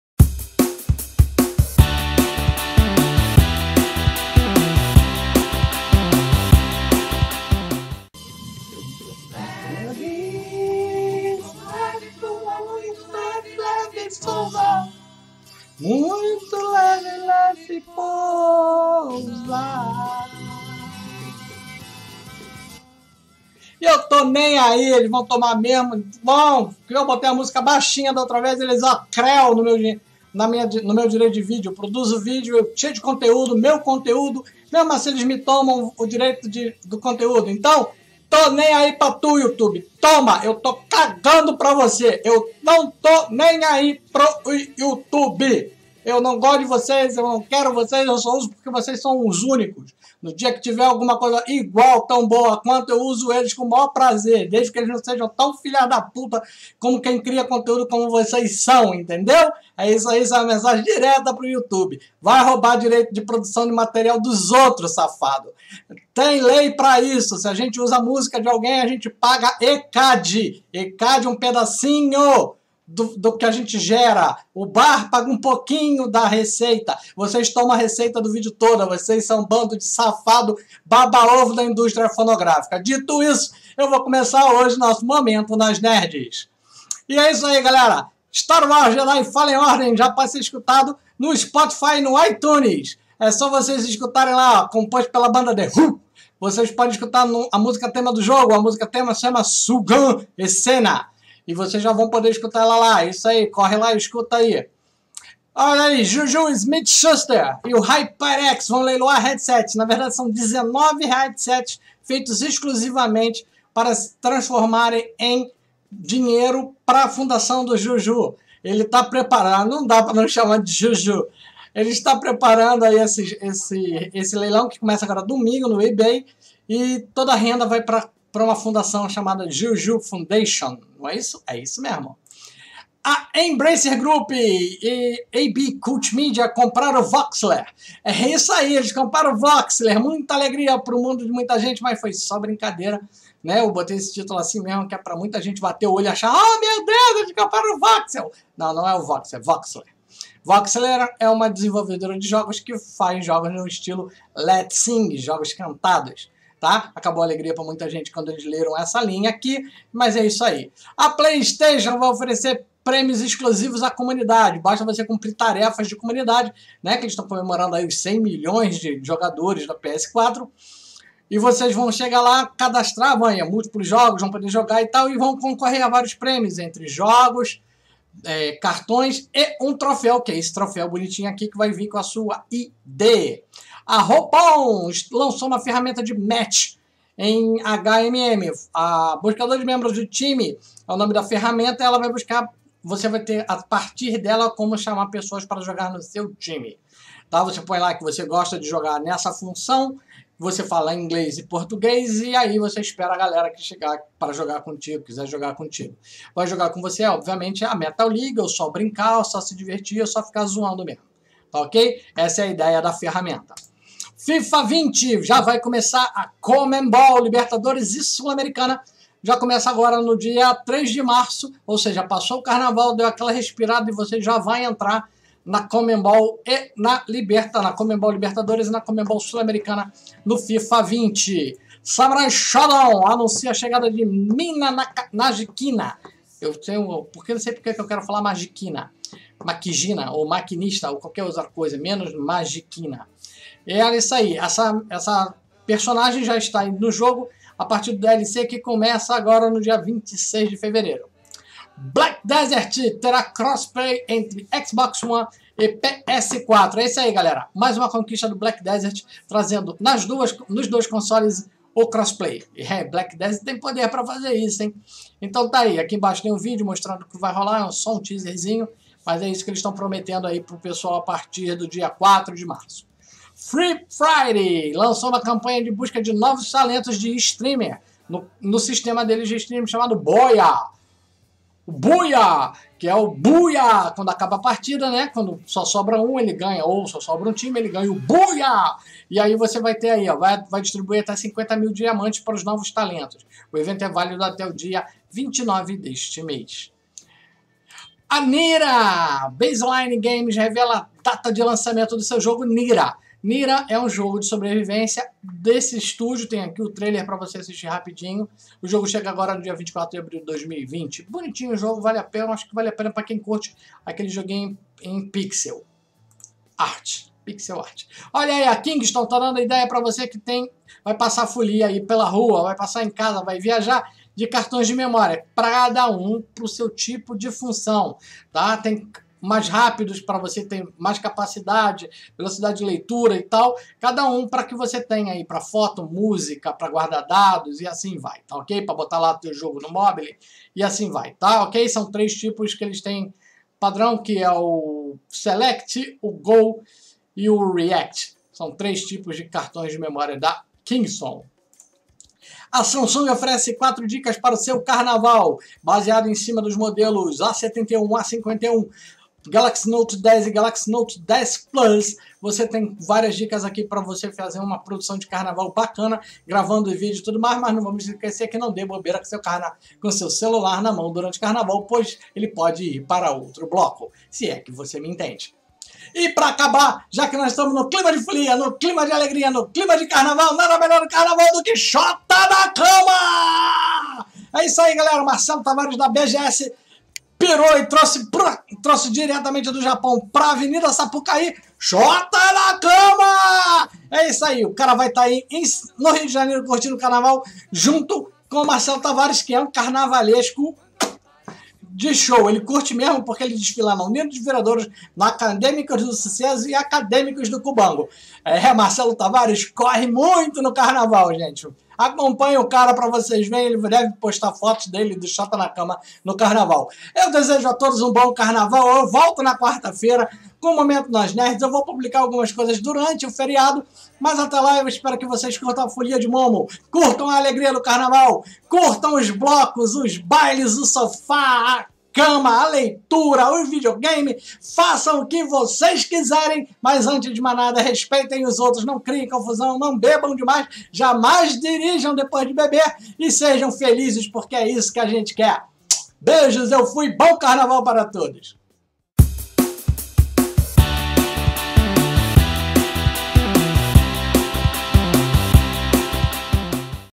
Momento Nós Nerds 20 – Nós Nerds
Duas vezes por semana estarei aqui lendo algumas das notícias que acho mais interessantes do Nós Nerds. As vezes dando minha opinião.